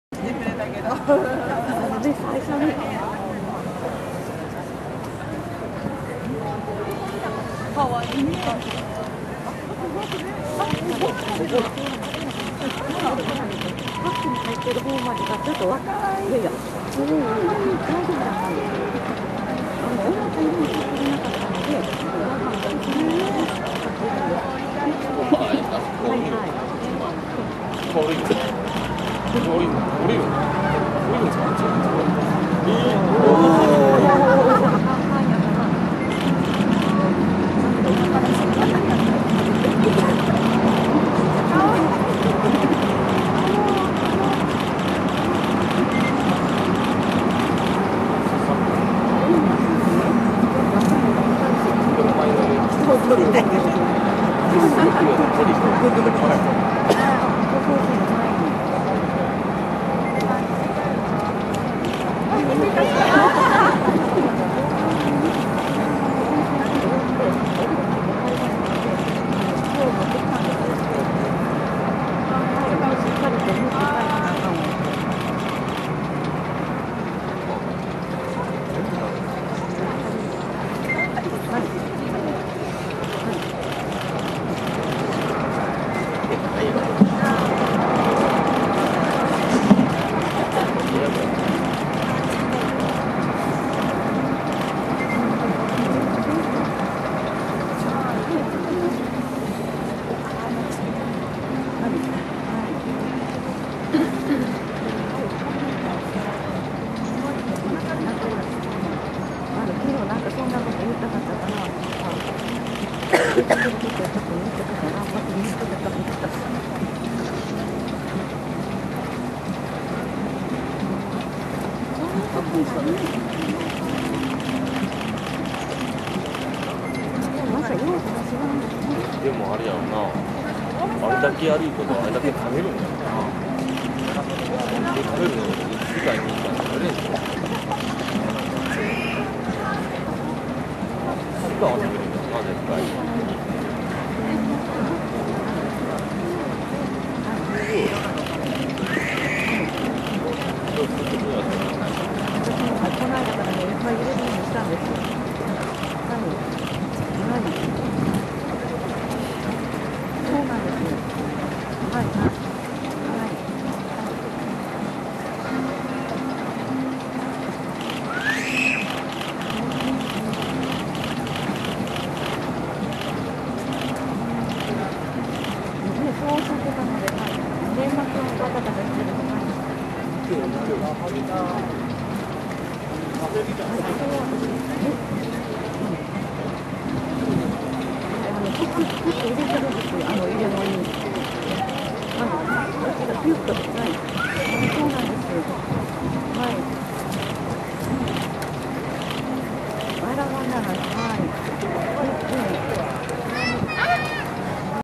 Giant Panda Twin Cubs Kaihin and Youhin of 2011 December at Wakayama, Japan #2-08
Youhin, Kaihin , twin babies born in Japan, 陽浜＆海浜 白浜 アドベンチャーワールド Youhin and Kaihin were 16-month old then.